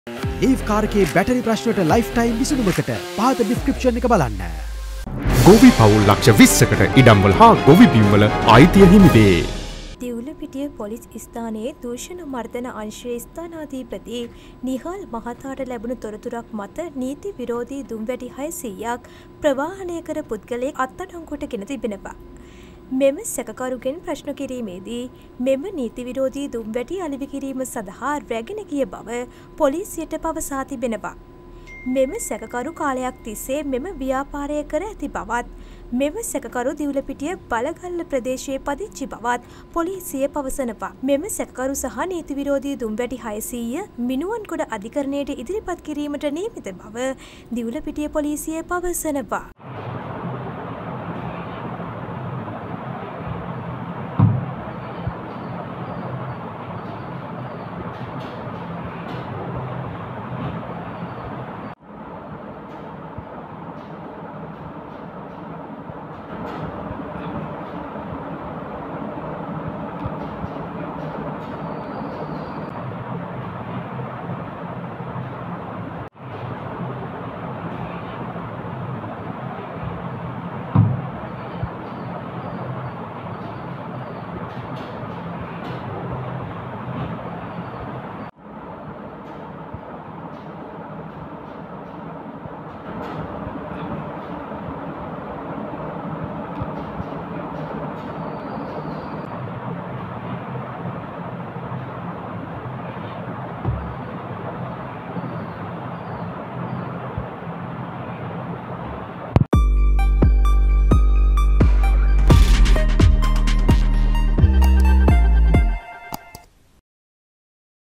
නීතිවිරෝධී දුම්වැටි 600 ක් ප්‍රවාහනය කර පුද්ගලයෙක් කොටු
වැරදි කළොත් දඩුවම් ලැබිය යුතුමයි
 නීතිවිරෝධී දුම්වැටි 600 ක්
ප්‍රවාහනය කර පුද්ගලයෙක් කොටු
කීයක්නම් අම්මලා අඬනවා ඇත්ද?